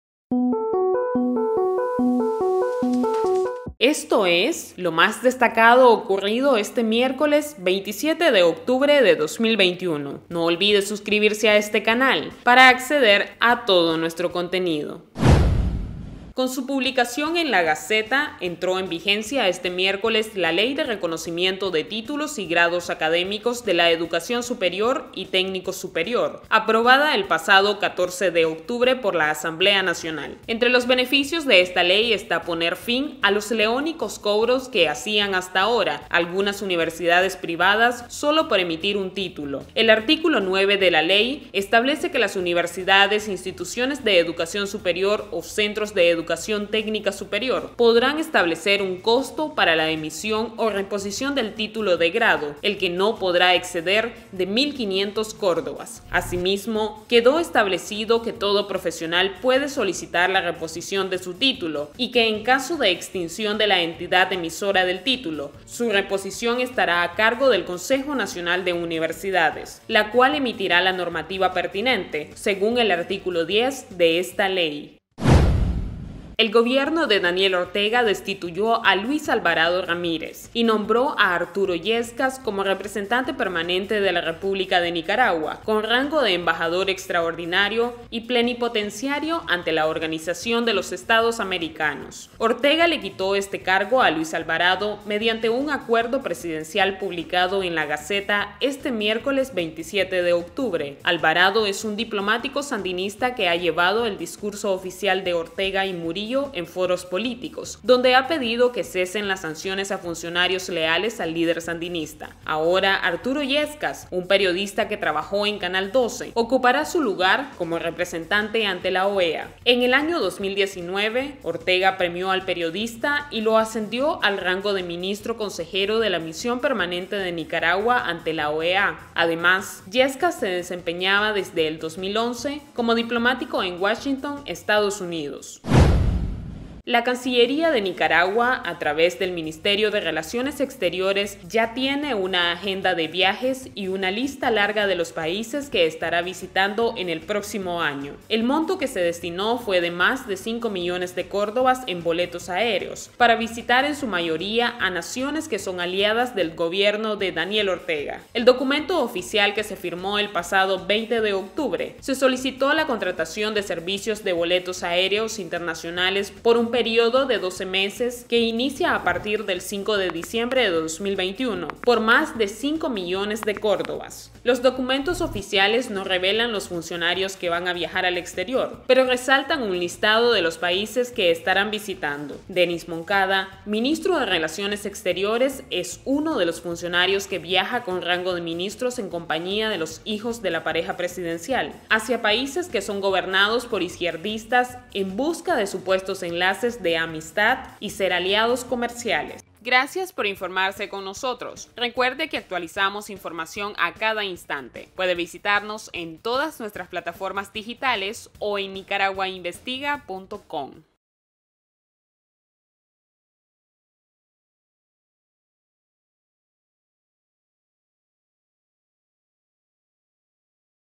LoÚltimo | ⚠🔺 Noticias de Nicaragua miércoles 27 de octubre de 2021
Estas son las noticias más importantes de hoy:
▶ Entra en vigencia ley que pone fin al excesivo cobro por la emisión de un título universitario
▶ Ortega destituye a Luis Alvarado como representante de la OEA y nombra a periodista
▶ Cancillería se receta millonario monto para viajes al exterior después de las elecciones

🎉 ¡Gracias por su preferencia, trabajamos para ustedes!